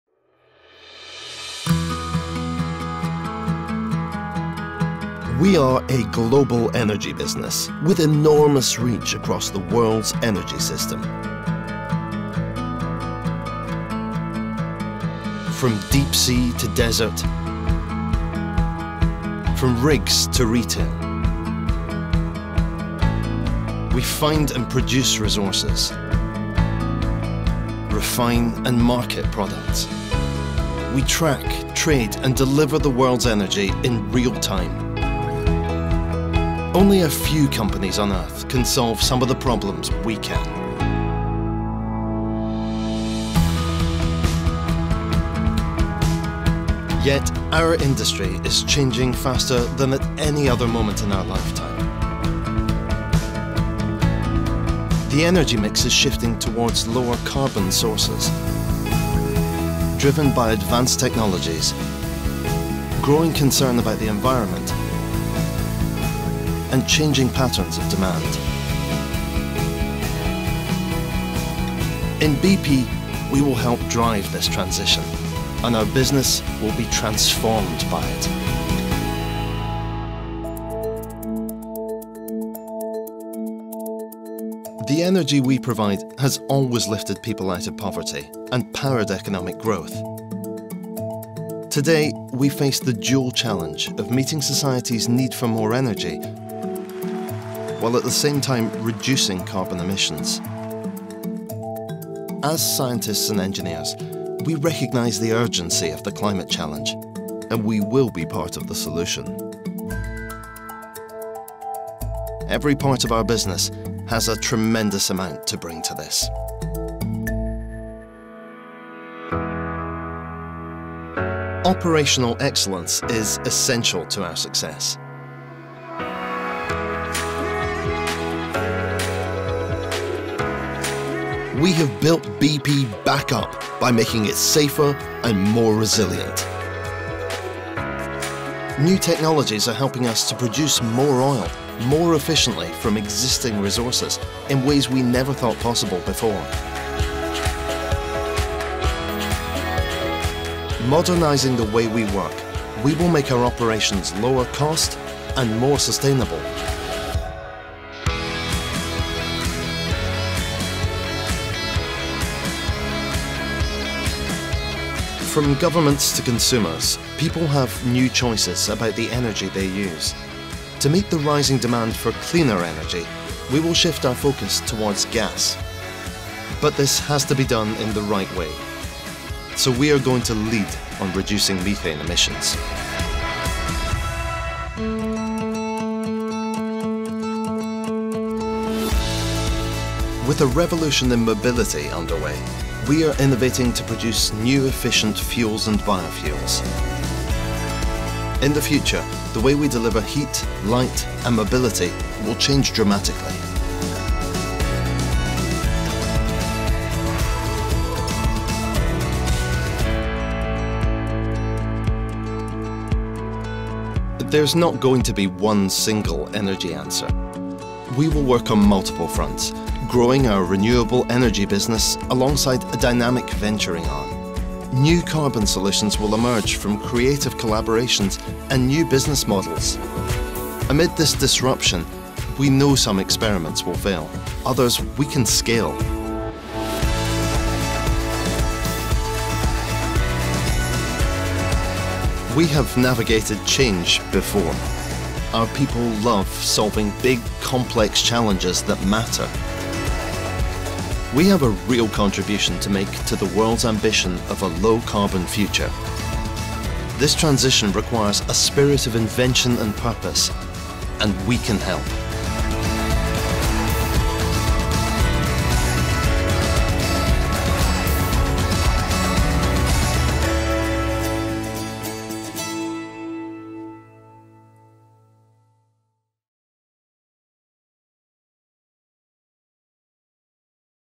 Advancing the energy transition
Watch how BP is advancing the energy transition in this special film